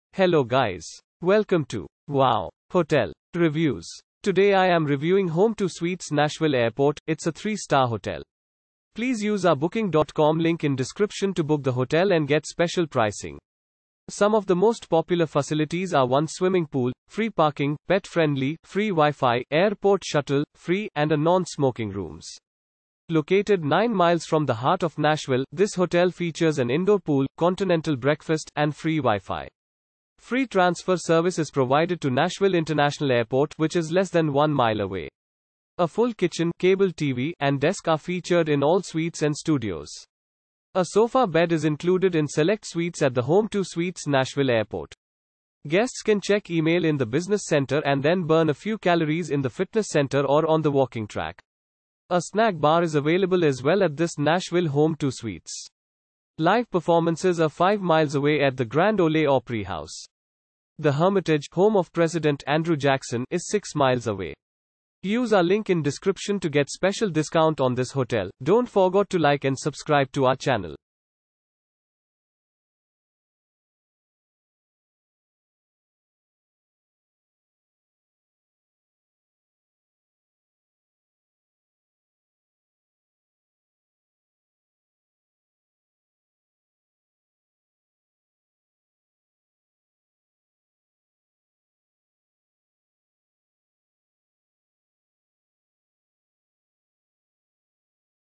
Home2 Suites Nashville Airport Review - Nashville , United States of America
Home2 Suites Nashville Airport

Located 9 miles from the heart of Nashville, this hotel features an indoor pool, continental breakfast, and free Wi-Fi. Free transfer service is provided to Nashville International Airport, which is less than 1 miles away.
A full kitchen, cable TV, and desk are featured in all suites and studios. A sofa bed is included in select suites at the Home2 Suites Nashville Airport.
Guests can check email in the business center and then burn a few calories in the fitness center or on the walking track. A snack bar is available as well at this Nashville Home2 Suites.
Live performances are 5 miles away at the Grand Ole Opry House. The Hermitage, home of President Andrew Jackson, is 6 miles away.